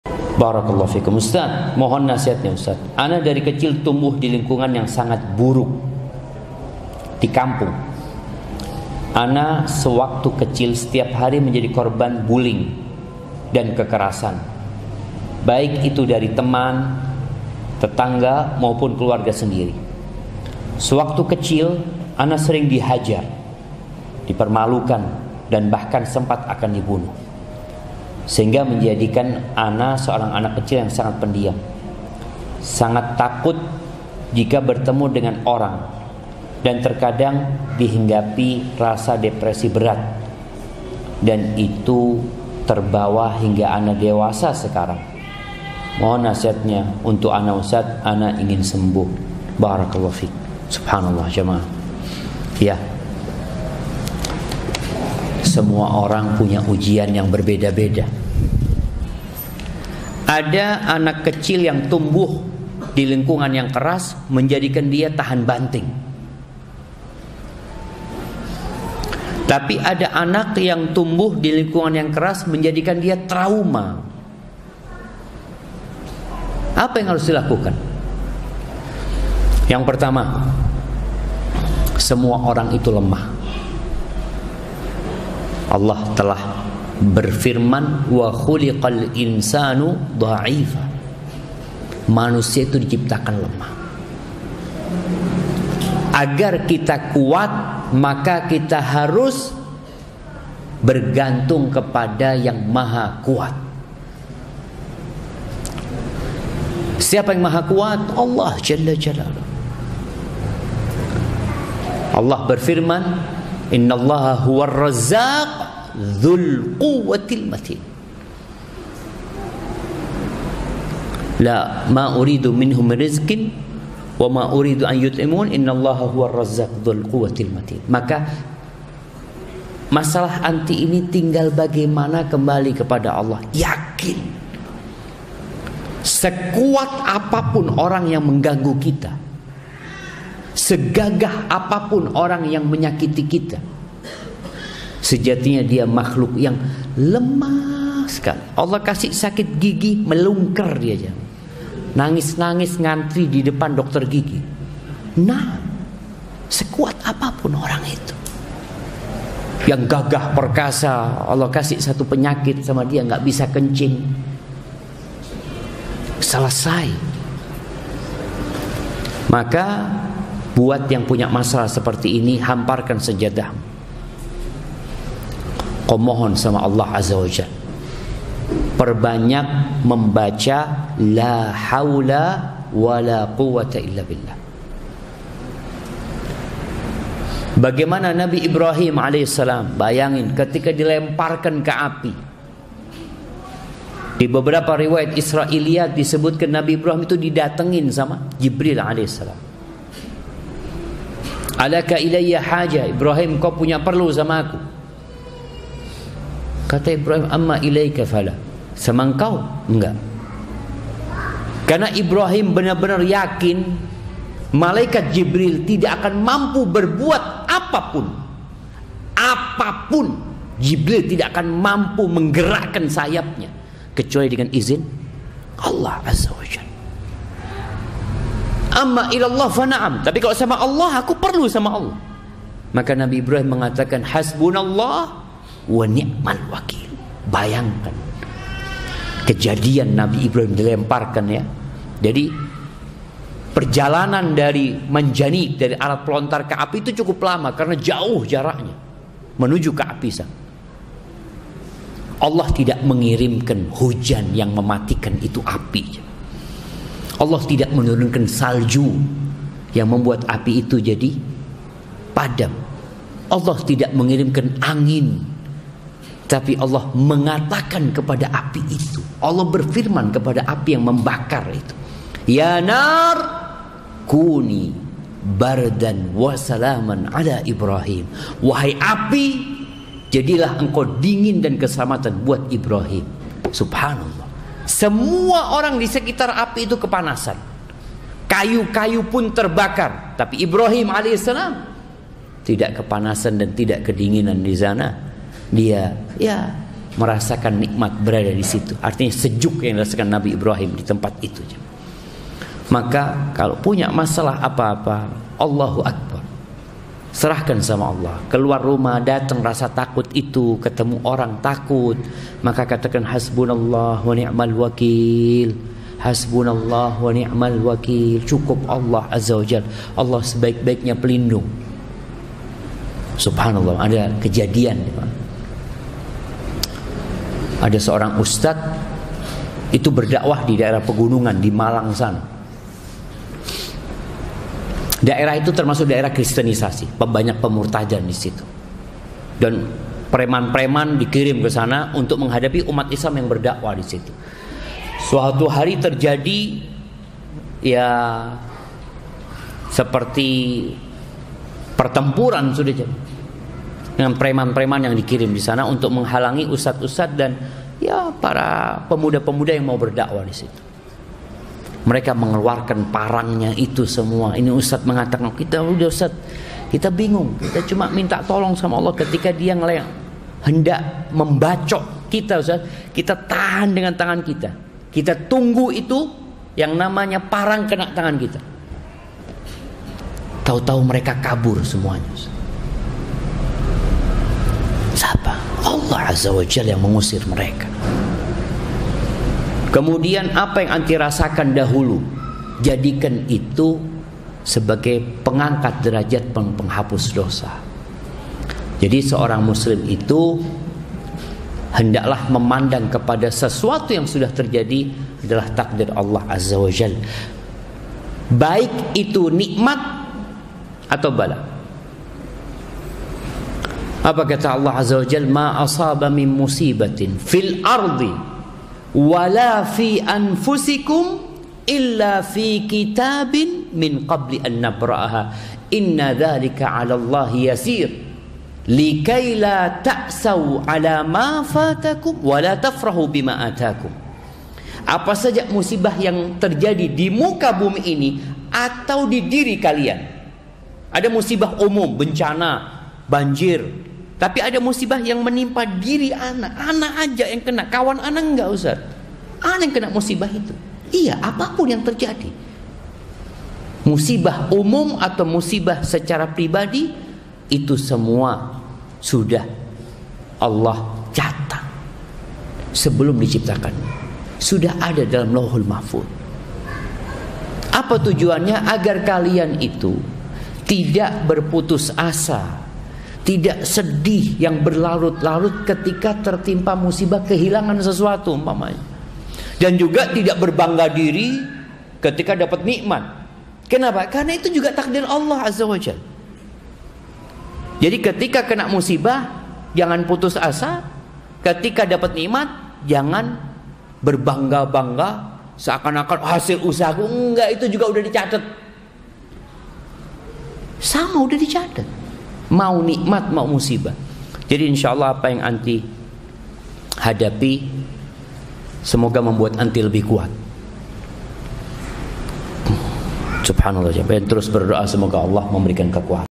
Saya Dulu Sering Dibully Hingga Merasa Depresi, Mohon Berikan Nasehat?
Ustadz Dr. Syafiq Riza Bsalamah, M.A.
Semoga bermanfaat ya...